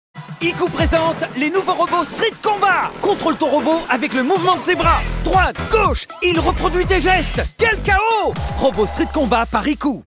Robots Street Kombat : vainqueur par KO !
Ils sont juste impressionnants ! C'est simple : tu donnes de vrais coups avec tes bras, à distance de ton partenaire, sans le toucher, OK ? Ton robot lui les reproduit pour mettre ton adversaire KO !
Comment font-ils ? Chaque Robot Street Kombat est équipé d'un système de détection de mouvements. Et pour ajouter de l'intensité aux combats chaque Robot Street Kombat YCOO de Silverlit émet des sons, des effets lumineux et peuvent combattre avec des accessoires pour augmenter l'impact !